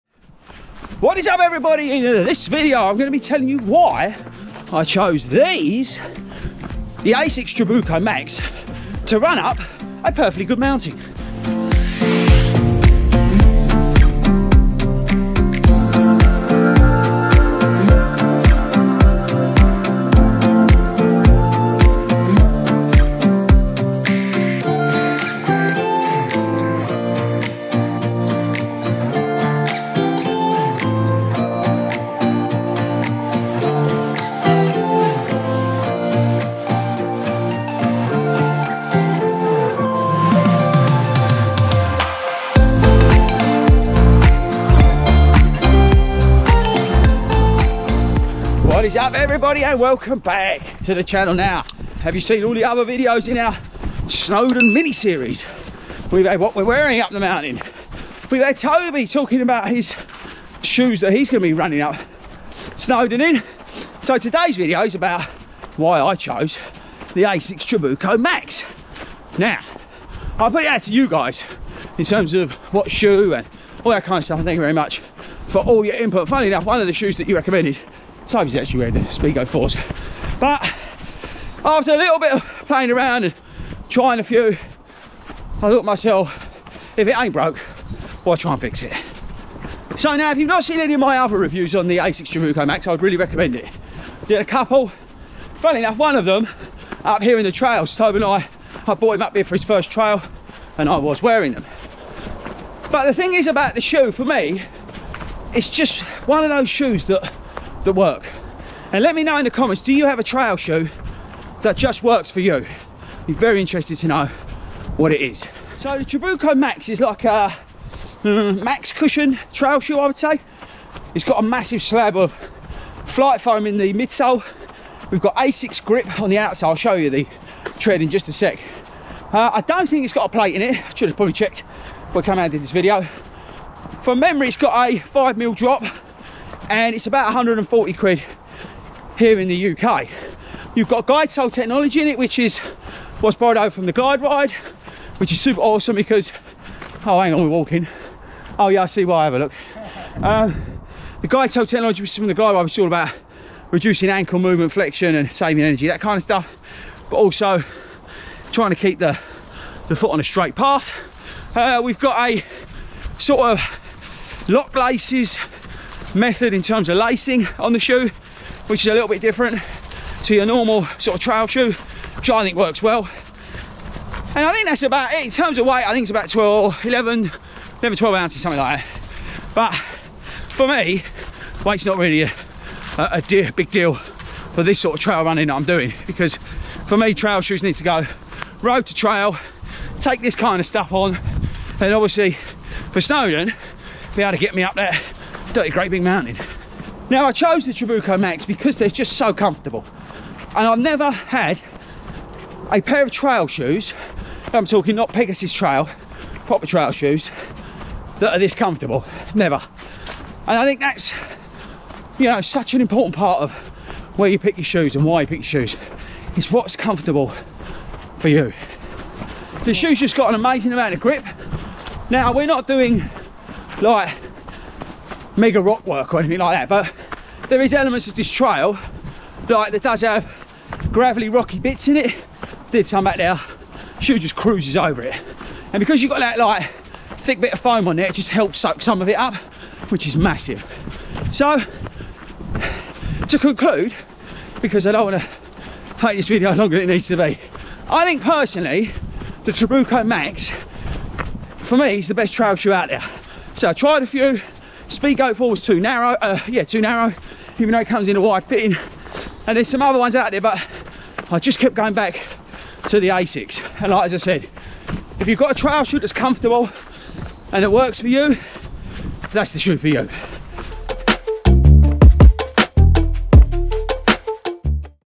My Trail Shoes - Asics Trabuco Max
My Trail Shoes - Asics Trabuco Max! In this video I tell you all about what I think is the best trail running shoe 2021 the Asics Trabuco Max!

TO DO'S!